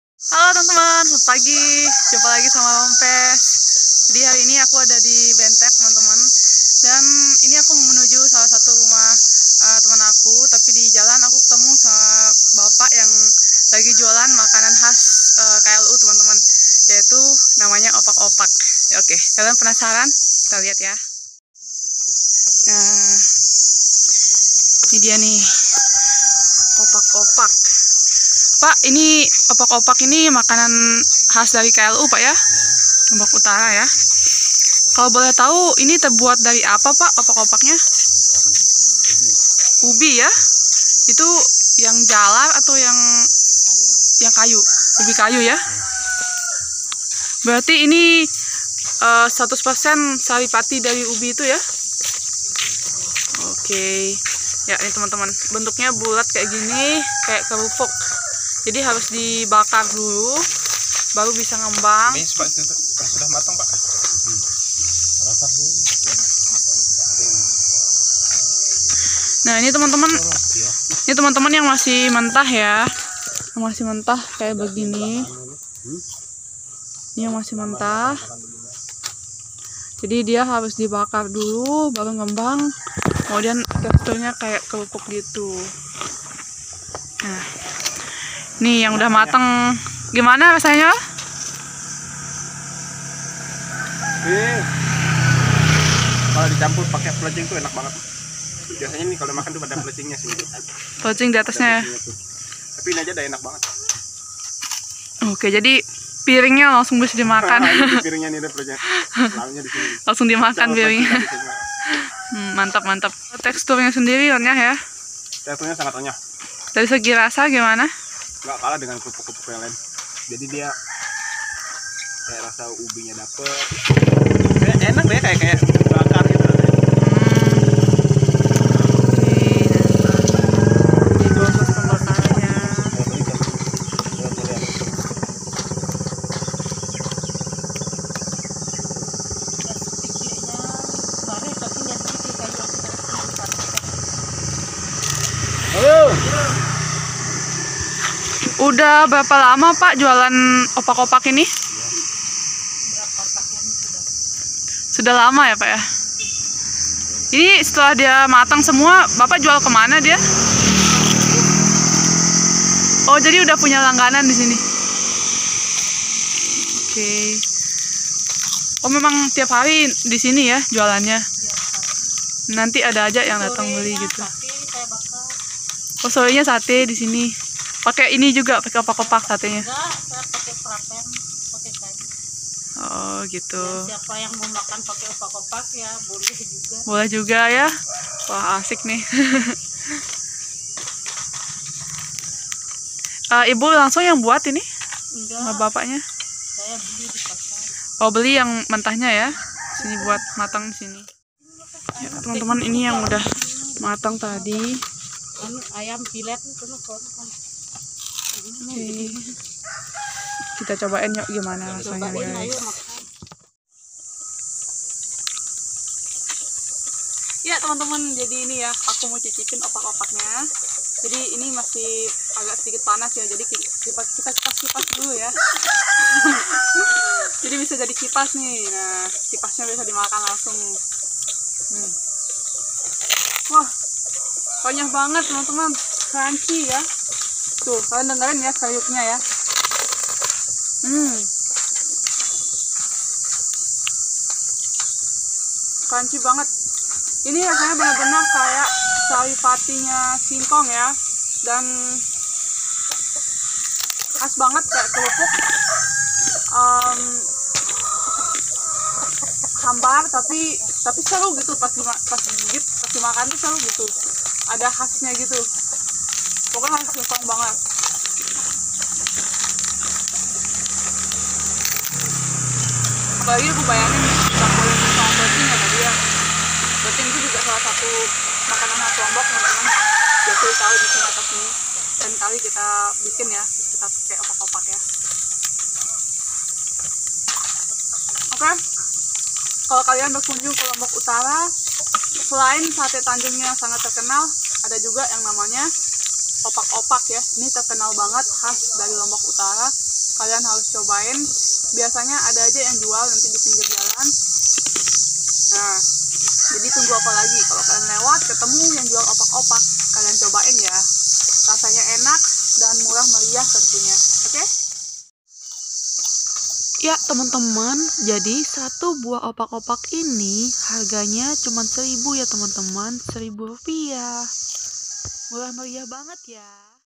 Opak-Opak Makanan Khas Lombok Utara - Rompes Cemara
Hallo sahabat Rompes... buat kalian yg berkunjung ke Lombok Utara, jangan lupa cobain nih Makanan khas KLU yang rasanya unik banget namanya Opak-opak. Terbuat dari sari pati singkong, sekilas opak opak ini bentuknya mirif dengan kerupuk tetapi jelas berbeda ya karena opak opak di kembangkan dengan cara di bakar ya teman-teman. tidak di goreng. Sangat sehat sekali. Harganya juga murah. Rata-rata dijual dengan harga Rp. 1000 saja/ opak opaknya. Kalian wajib coba ya.. Rasanya Khas Banget.😍😍